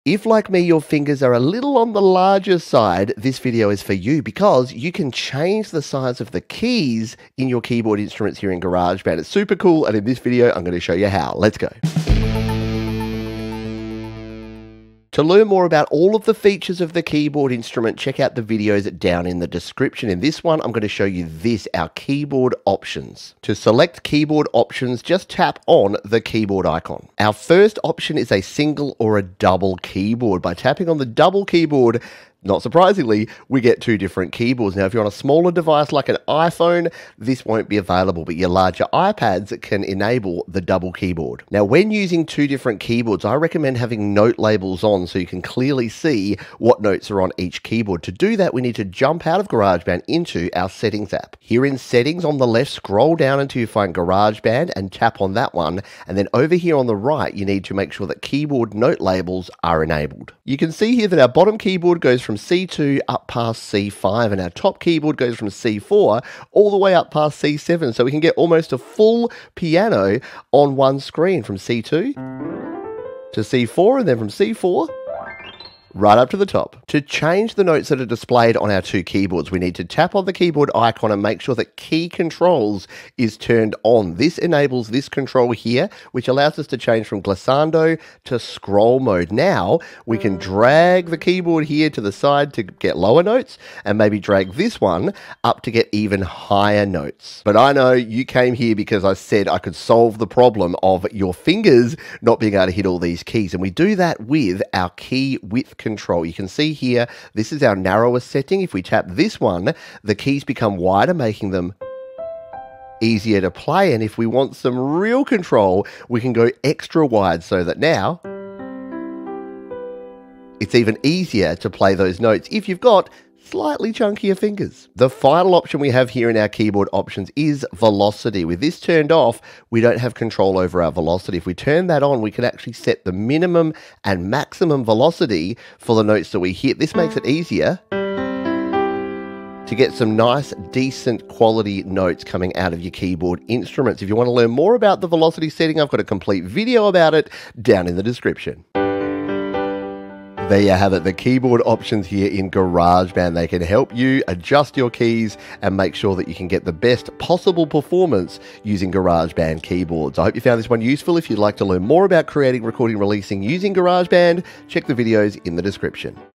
KEYBOARD OPTIONS in GarageBand iOS (iPad/iPhone)
⏰ Time Stamps:
00:00 Keyboard Options
00:27 Single/Double Keyboard
00:42 Note Labels
01:16 Change Notes Displayed
01:37 Key Width
02:04 Velocity Control